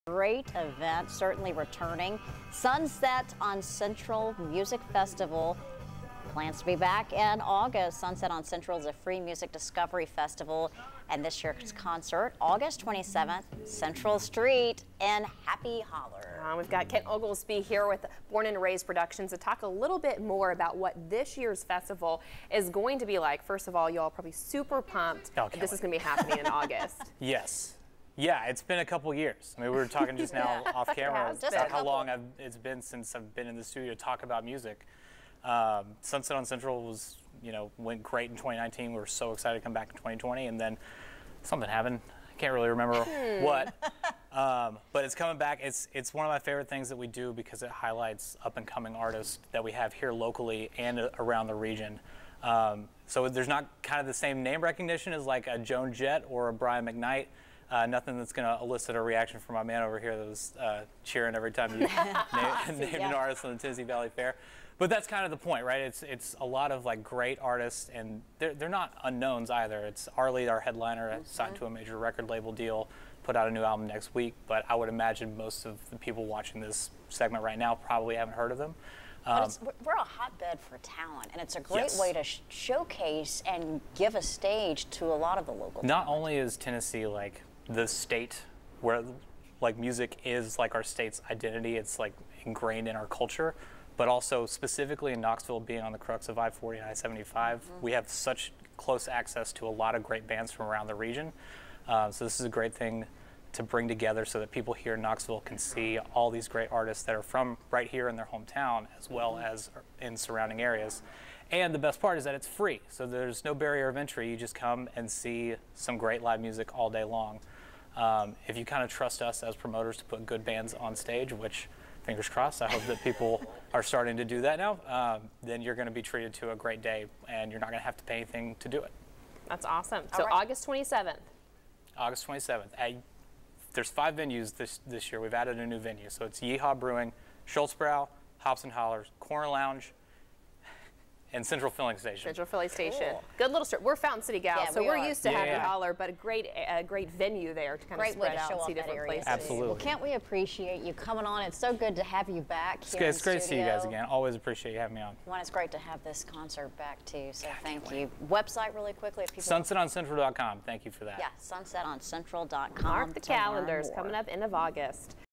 Sunset on Central Concert set for August 27
Sunset on Central is a free outdoor concert set for August 27, 2022 in the Happy Holler area of Central Avenue. Visit sunsetoncentral.com for more . June 8, 2022-4pm